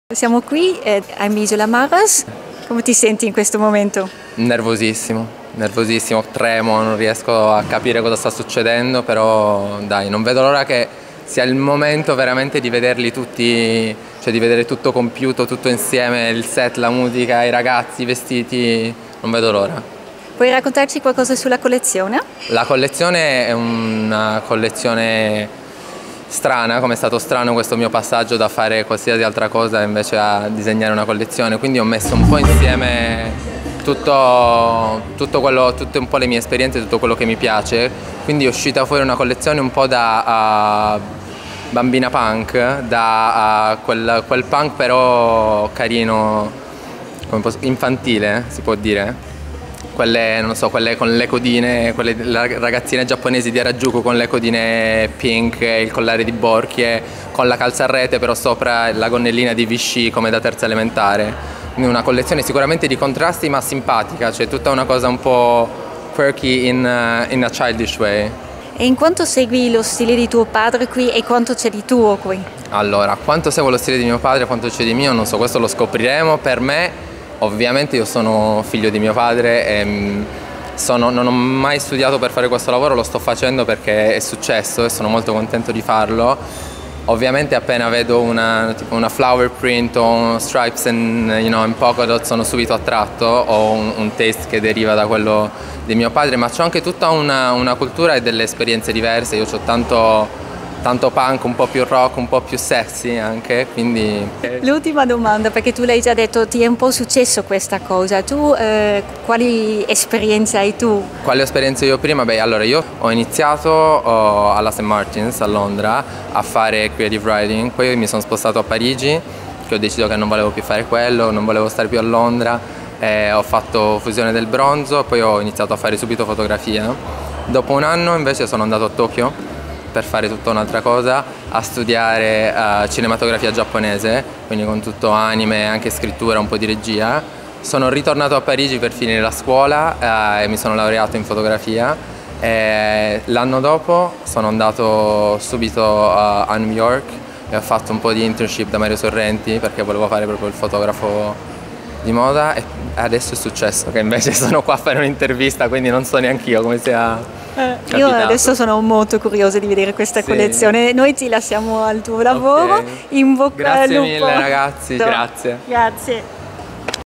I'm Isola marras Estate 2018. Il debutto di Efisio Marras come Direttore Creativo
Un party per celebrare il debutto di Efisio Marras, l'omonimia non è un caso of course, nuovo Creative Director di I'm Isola Marras. Una collezione - anzi una pre-collezione - uomo e donna Primavera Estate 2018 che è tutta all'insegna dell'innovazione, estremamente contemporanea e per molti versi rivolta al futuro, e rivolta a mondi e generazioni molto diverse tra di loro. Leggi tutto su ADVERSUS